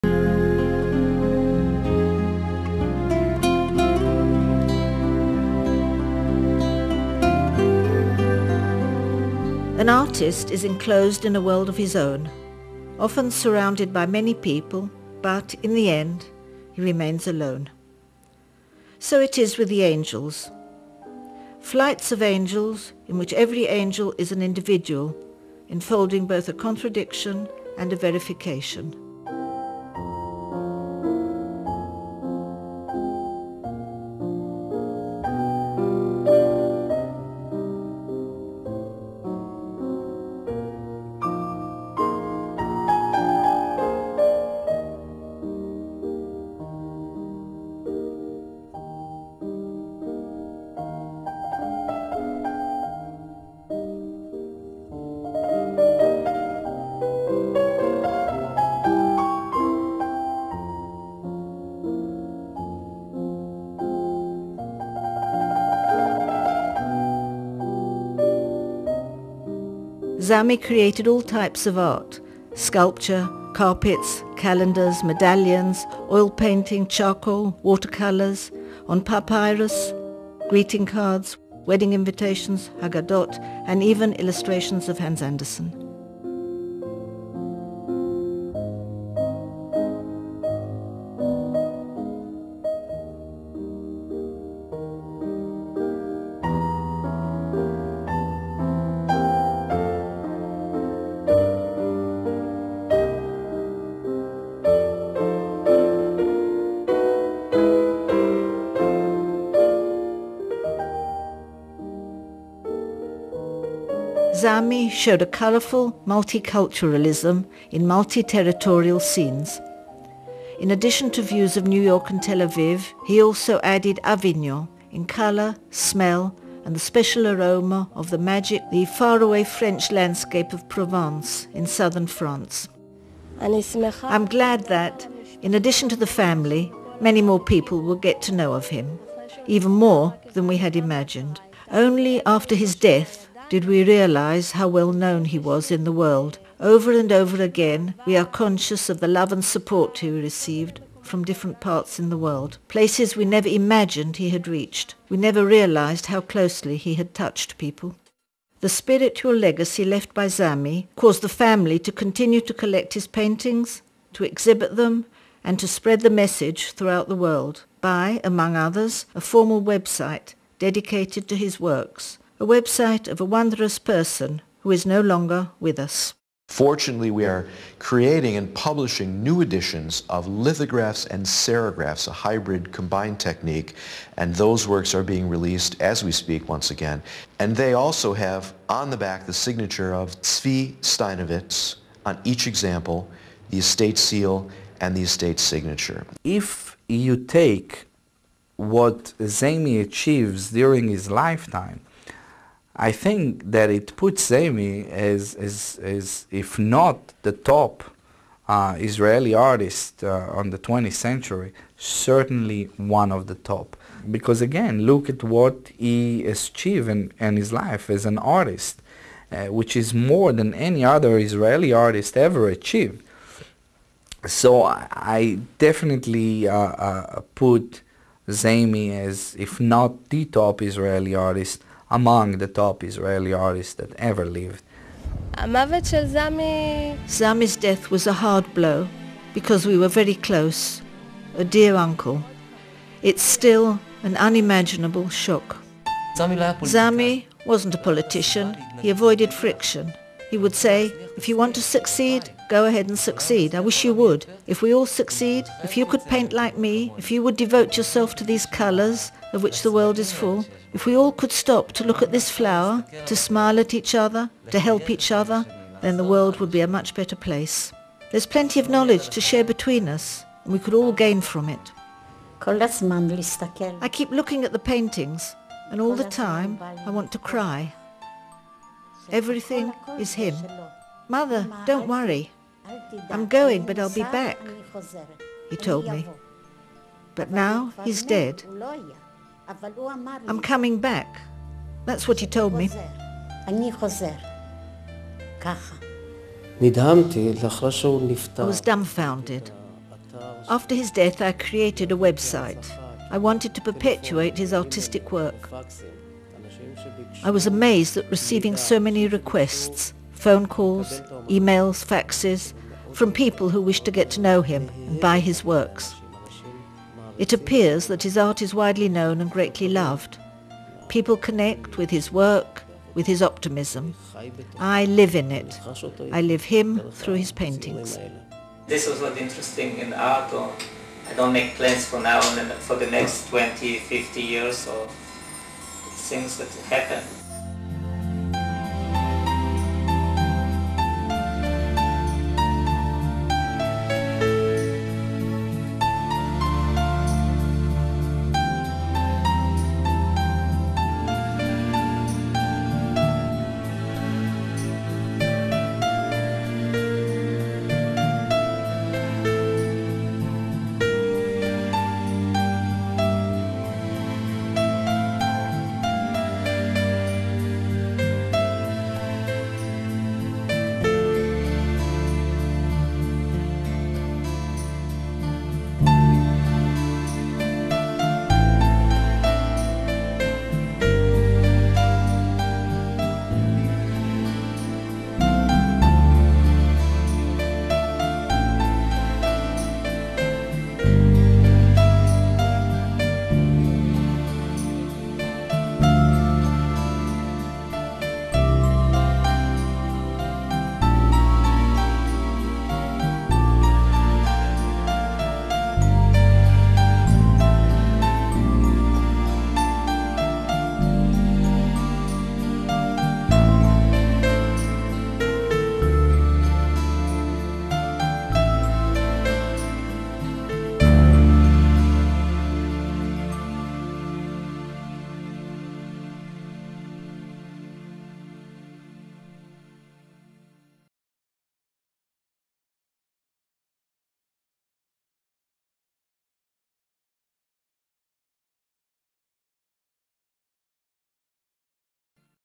The Authobiography Movie of Zamy Steynovitz part C.avi
BIOGRAPHY
ZAMY STEYNOVITZ

Zamy Steynovitz was born in Liegnitz, Poland in 1951and at a very early age he aspired to be a painter. He won first prize in an art competition for children before immigrating to Israel in 1957.

Formally educated at the Art School in Tel-Aviv and the Royal Academy in London, he completed his studies and began artistic pursuits in earnest. Zamy established his place in the art world after displaying his work in one-man exhibitions and art fairs around the world.

Zamys art displays chromatic and thematic richness and his choice of subjects has been strongly influenced by Jewish tradition and folklore. Additionally, his work presents general themes such as Paris cafes, still lives, flowers, circuses and landscapes.

In the early stages of his work, he used rich pastels and light brush strokes. When he visited South America in the early 1980s, his work reflected his new surroundings and were further enhanced by local brightness and colorfulness. His art gained chromatic power; the palette became richer in tones, the textures thicker, and the background darker and more colorful. Together with these changes, the thematic persistence in his creation was maintained, allowing him to develop into a sensitive and mature artist.

Zamys paintings are a reflection of his Eastern-European Jewish heritage, and they are enhanced by a rich choice of warm tones and colors. He evoked a universal humanistic idea in his creation; mans connection to his heritage and physical surroundings. These are two imperatives aspects in our lives, which during these estranged technological times we should recall.

As a result of his devotion to world peace, he became known in the circles of the Nobel Institute for Peace in Norway, and consequently was acquainted with many Nobel prize winners, such as Anwar Sadat, Menahem Begin, the Dalai Lama, Itzhak Rabin, Shimon Peres, Elie Wiesel, Desmond Tutu, Oscar Arias Sanchez, as well as many of the worlds greatest leaders and artists.

Zamy tragically passed away in September of 2000.